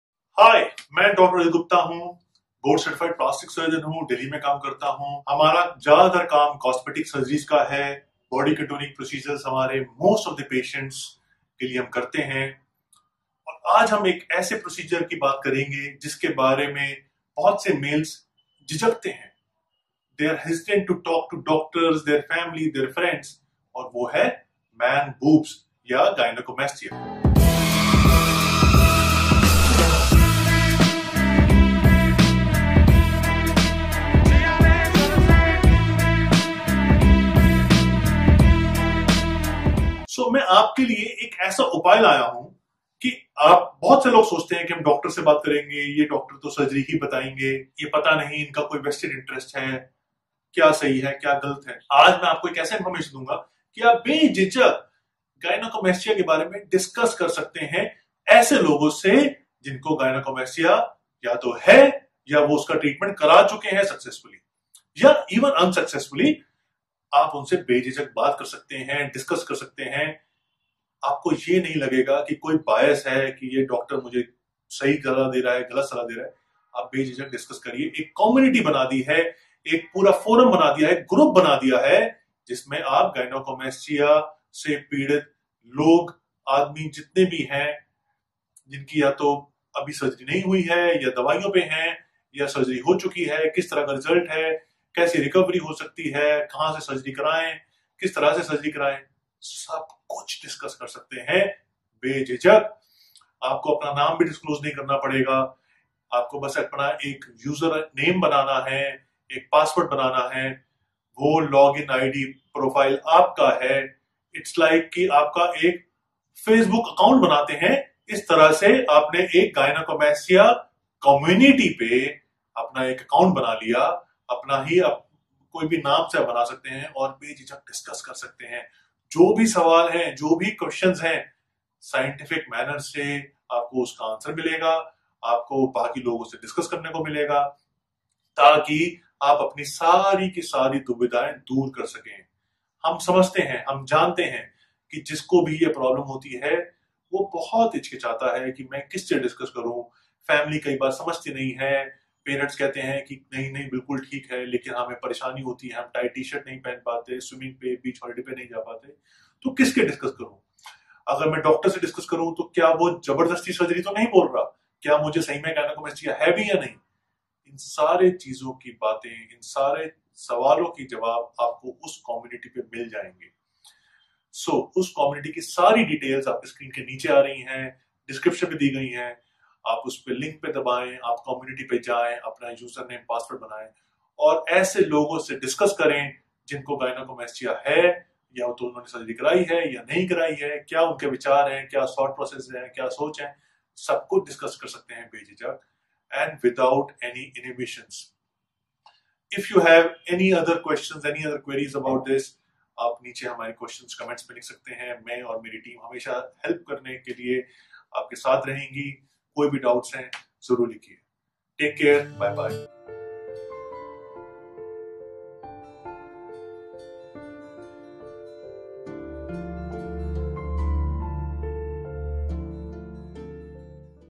Discuss Gynecomastia Without any Hesitation in India’s First Ever | Dr Rajat Gupta - Excel Hospital
Alone we can do so little; Together, we can do so much!
Dealing with Gynecomastia could be very challenging. People suffering from this issue are very hesitant to talk about Man Boobs with their family, friends or even doctors. They feel that their family and friends might not understand them or judge them, and doctors would right away suggest surgery without listening to them. During my years of practice, I have witnessed a lot of hesitation to talk from men to talk about this issue openly.

Therefore, the team of RG Aesthetics brings to you the first-ever GynecomastiaCommunity in India! An Initiative for Patient Education and Interaction.

The purpose of this community is to help provide people suffering from Gynecomastia an opportunity to talk out their worries and fear with surgeons and patients (who have been treated or are getting treated presently). You can find answers to anything related to Gynecomastia in this community like recovery, results, treatment, etc.

All you have to do is sign up and make your user account. You can choose to not disclose your name.
✅ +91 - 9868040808, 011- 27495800, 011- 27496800